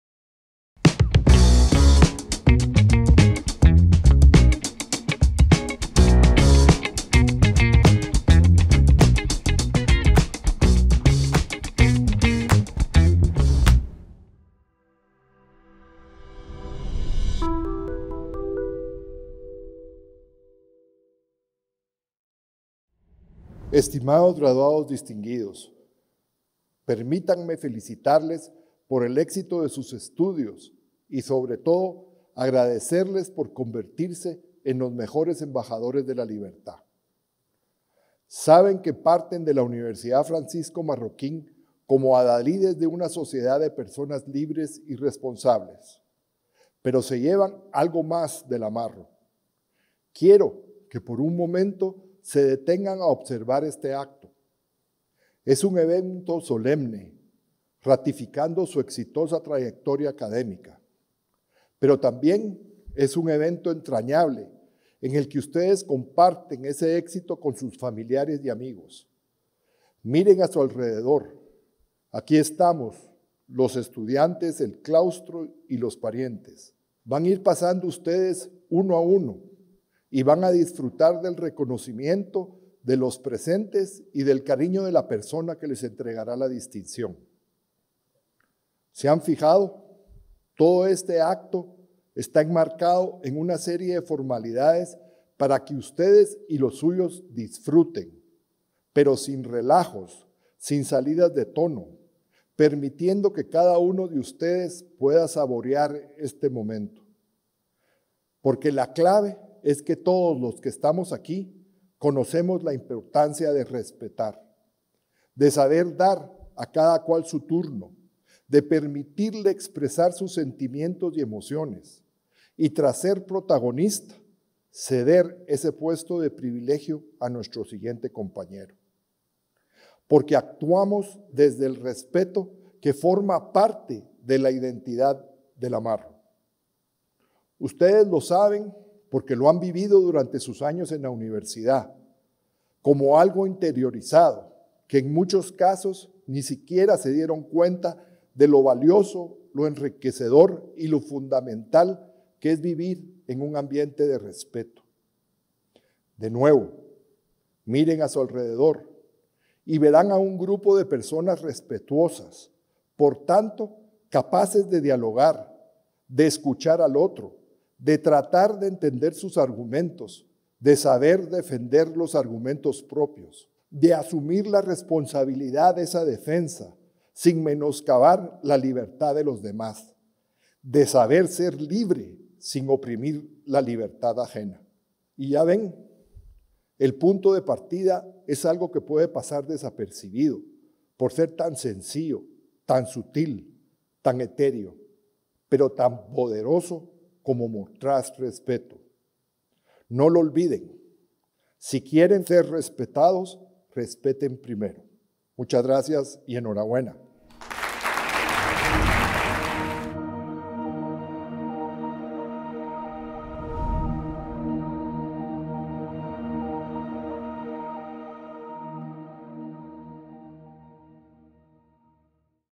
Discurso del Rector durante el Acto en Honor de los Graduandos Distinguidos (Noviembre 2023)
El rector Ricardo Castillo expresó sus felicitaciones al grupo de profesionales egresados de la Universidad Francisco Marroquín, durante el Acto en Honor de los Graduandos Distinguidos el 10 de noviembre del 2023 por sus méritos académicos obtenidos. En su discurso destaca la importancia de su papel como embajadores de la libertad y la responsabilidad.

“Porque actuamos desde el respeto que forma parte de la identidad de la UFM, ustedes lo saben porque lo han vivido durante sus años en la universidad como algo interiorizado”.

El rector destaca la naturaleza solemne y sincera del evento, donde los graduados son reconocidos por sus logros académicos junto a catedráticos, familiares y amigos. Enfatiza la importancia del respeto, reflexionando sobre el valor de vivir en un entorno de libertad que fomenta el diálogo y la comprensión de los argumentos de los demás.

“El punto de partida es algo que puede pasar desapercibido por ser tan sencillo, tan sutil, tan etéreo, pero tan poderoso como mostrar respeto. No lo olviden, si quieren ser respetados, respeten primero”.

Finalmente, Ricardo Castillo, anima a los graduados a practicar el respeto como un principio fundamental para el crecimiento personal, social y profesional.

Organizado por:
Universidad Francisco Marroquín